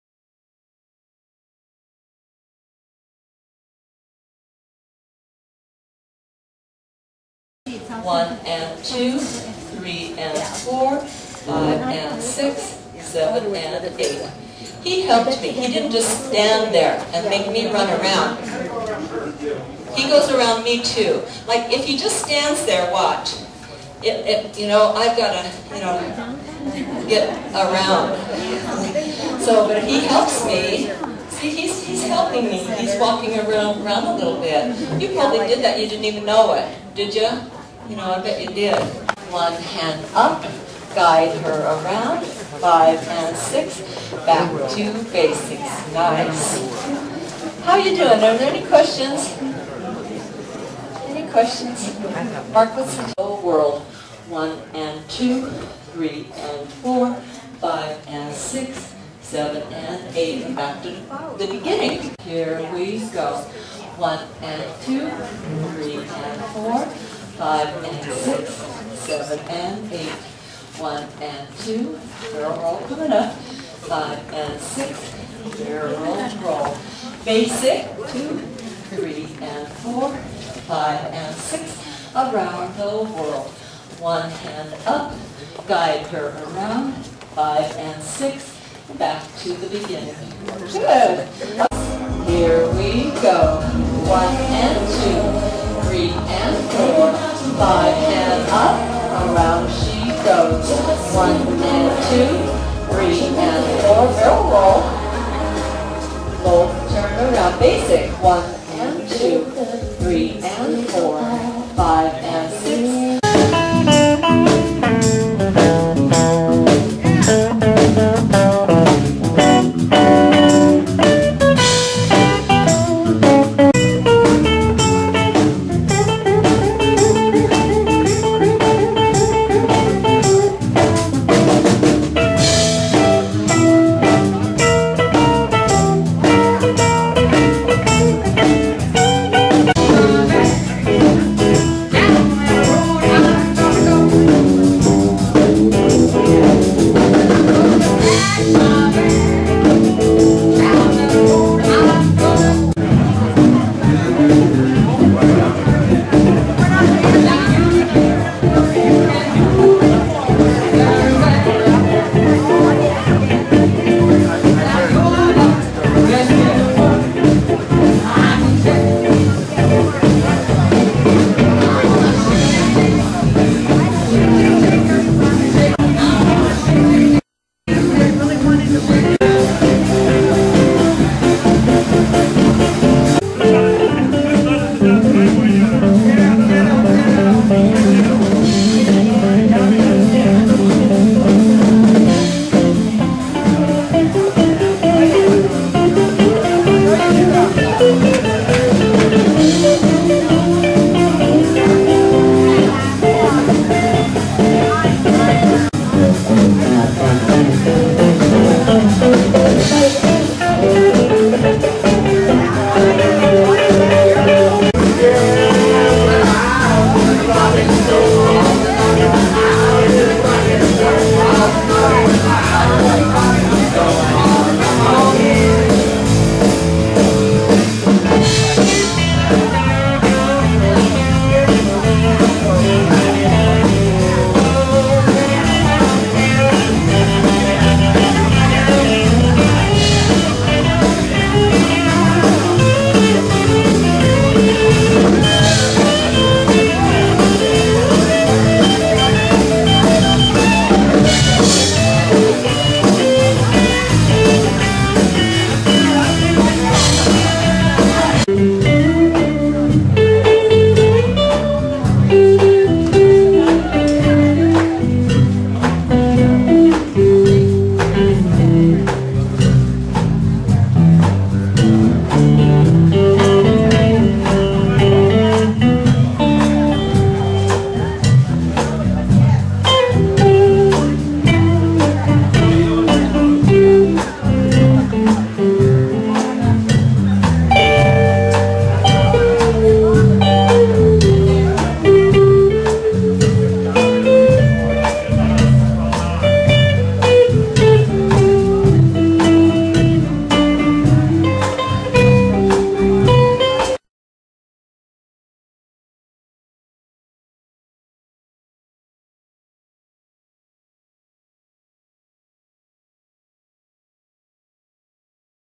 MAIA SANTELL & HOUSE BLEND
On Sunday April 12, instead of the usual Harbor Happening, I've turned it over to Tony to put on his own special EASTER DANCE at China Harbor. Arrive at 7 PM for a $5 Nightclub Two-Step lesson with PATTI CONLEY & MARC IMLAY. Then dance to live music with MAIA SANTELL & HOUSE BLEND from 8 to 11 PM. They will play lots of Soulful Blues and Jazz great for dancing West Coast Swing and more. Dance admission is $10.